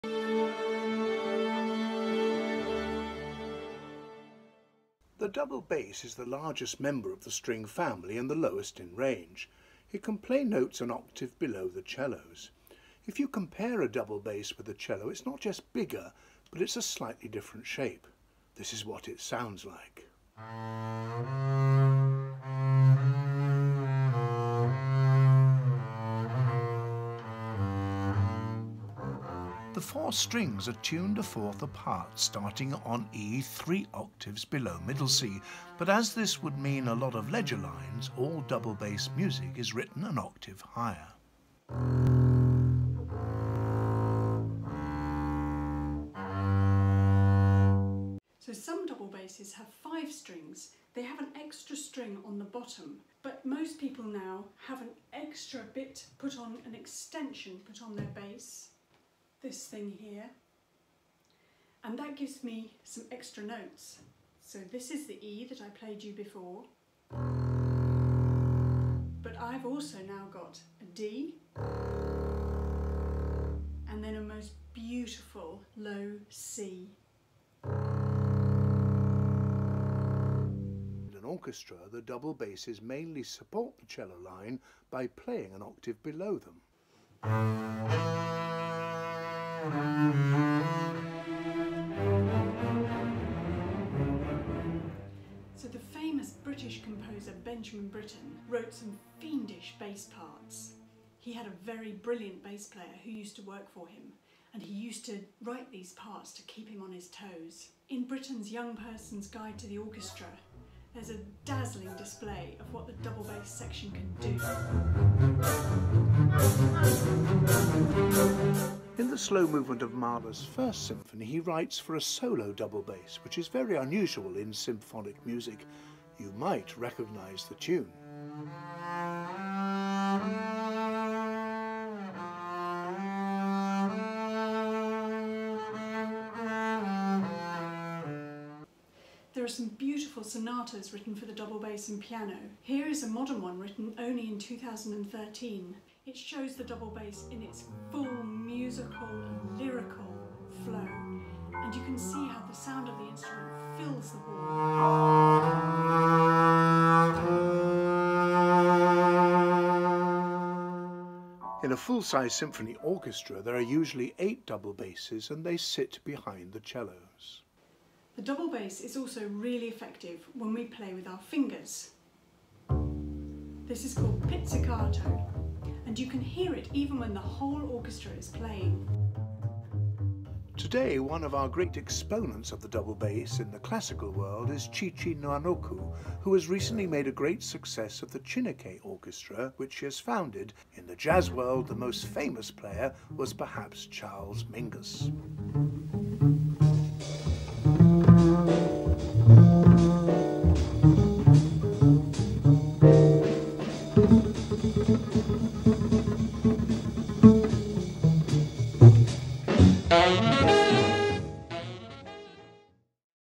The Double Bass
An introduction the double bass, presented by NLO bassist Lucy Hare & conductor Ronald Corp. Repertoire includes extracts from music by Beethoven, Britten, and Gubaidulina.